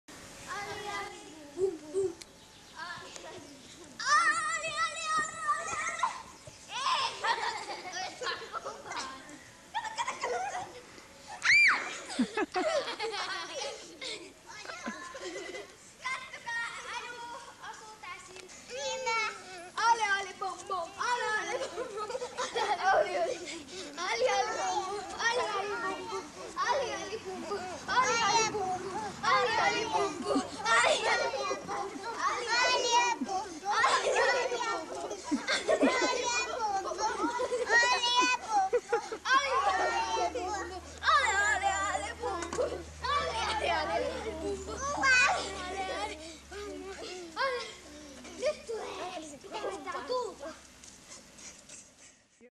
Anu vankina
Isommat kiusaa Anua. 1994 Laaksolahdessa.